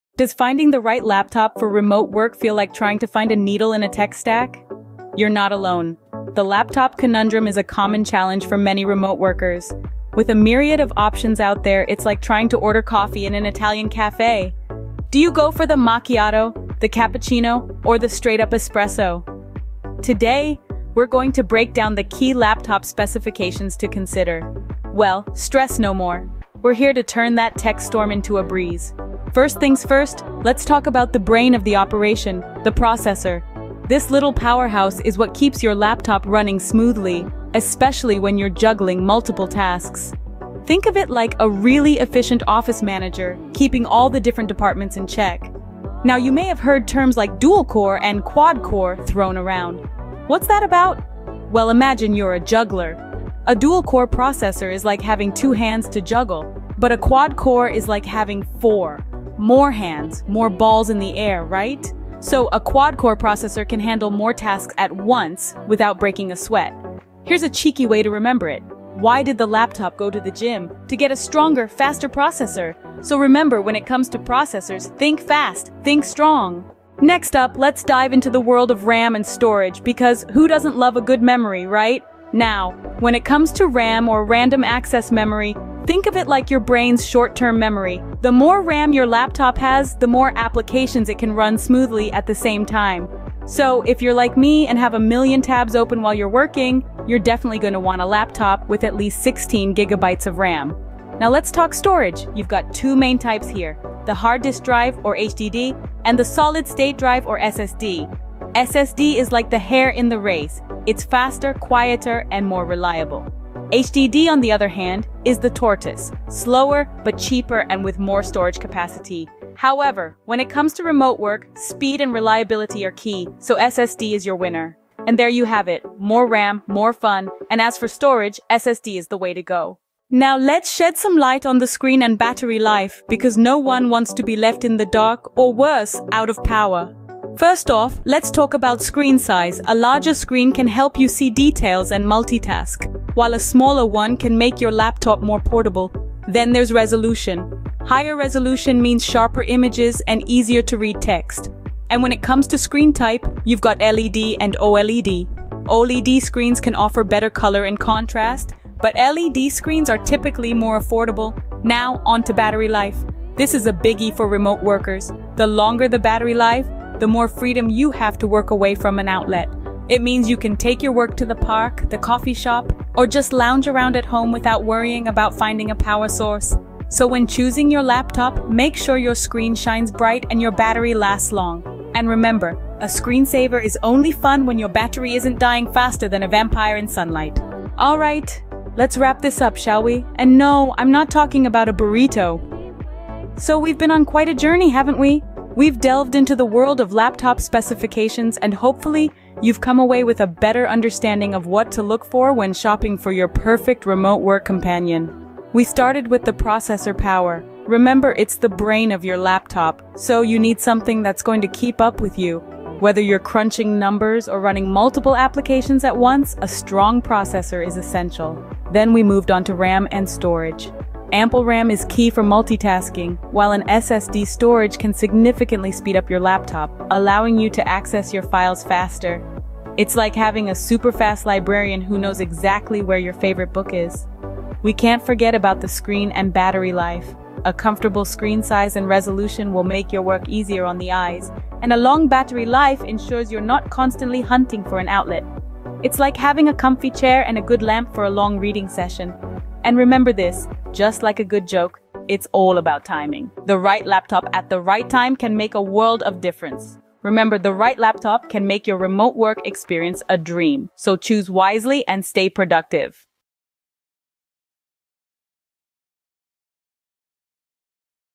Laptop Specs: Remote Workers' Ultimate Guide
In this video, we delve into the world of remote work and explore the best laptop specifications for remote workers. From tips to recommendations, we've got you covered with all you need to know to maximize your productivity and efficiency. And yes, a dash of humor is included to keep you entertained along the way!

Whether you're a freelancer, digital nomad, or remote employee, having the right laptop can make all the difference in your work-from-home setup. Join us as we break down the essential specifications you should look for, including processors, RAM, storage, display, and more.

So sit back, relax, and let us guide you through this informative and entertaining journey into the realm of remote work technology. Let's optimize your remote work experience together!